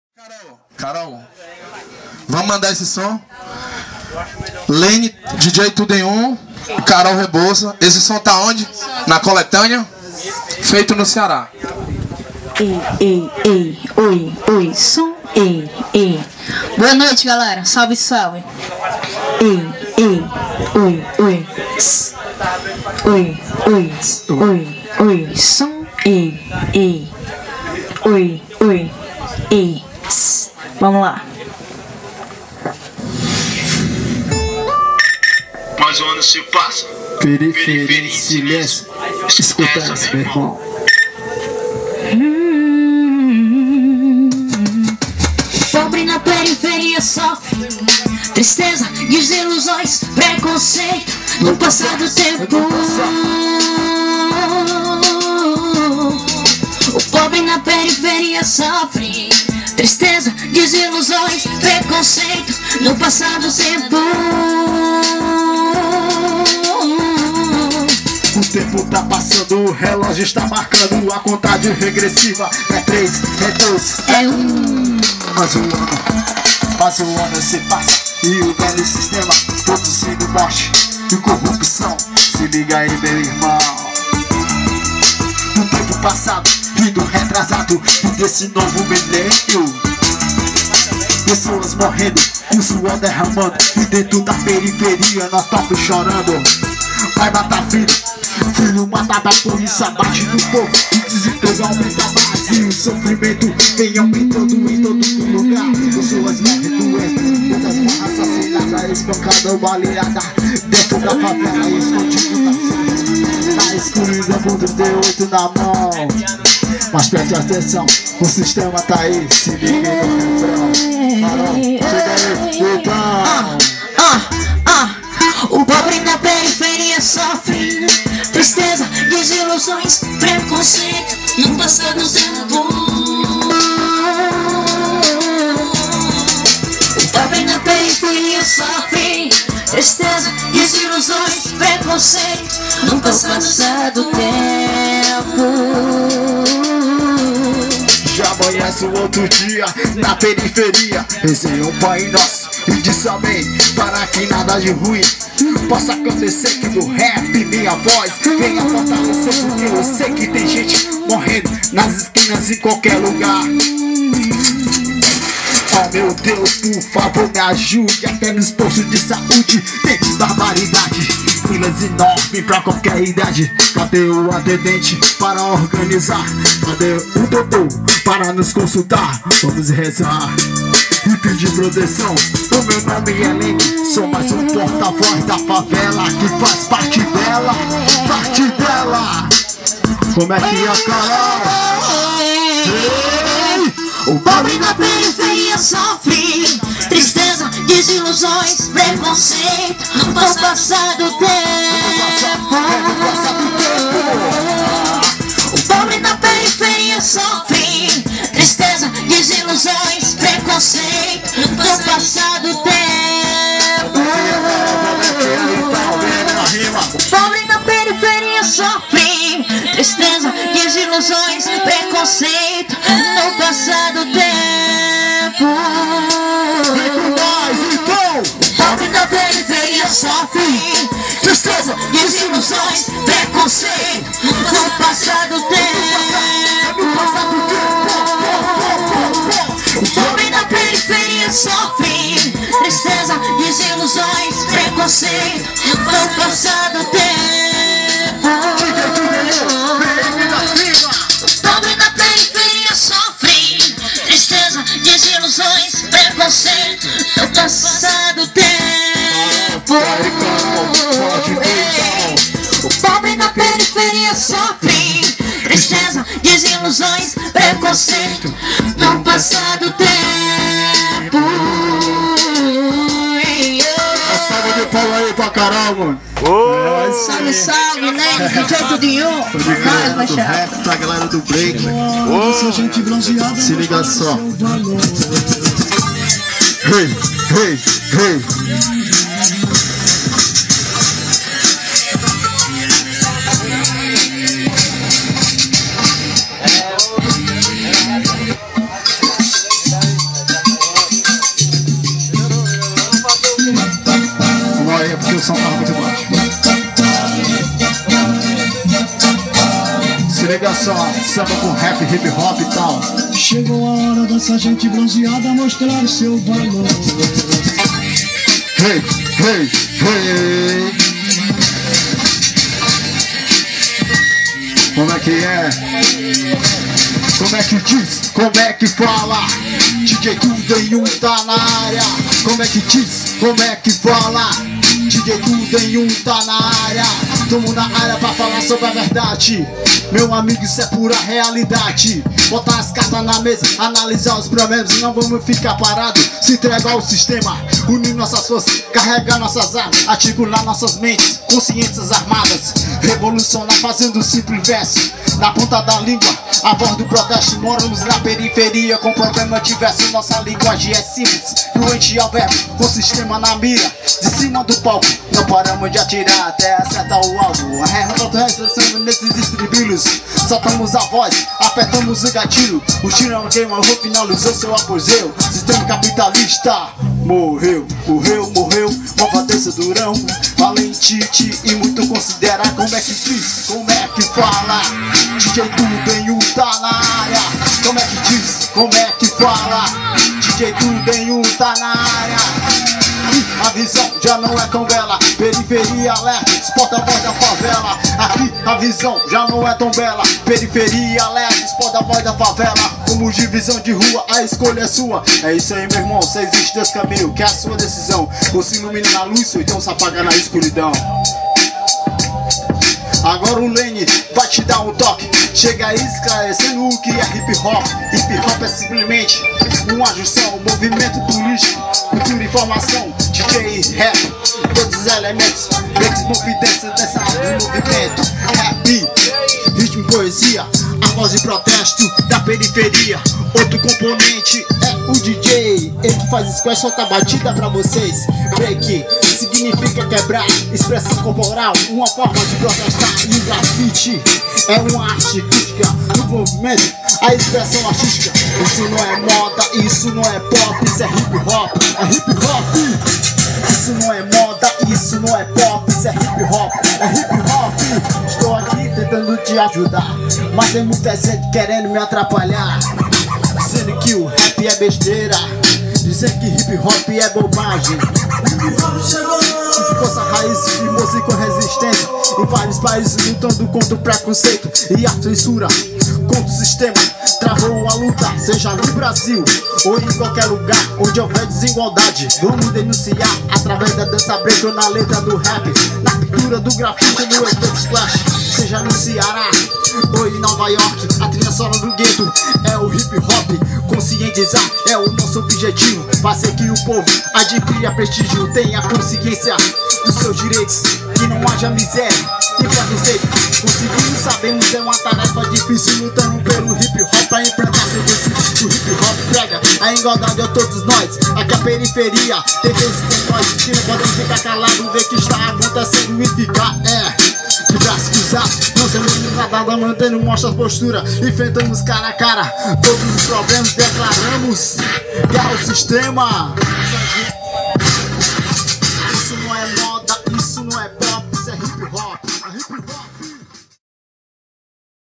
LENNY FERNANDES NA 5ª EDIÇÃO DO CORREDOR SONORO
Lenny Fernandes mais conhecido como TUDO EM 1 foi o homem da estrutura na 5ª edição do Corredor Sonoro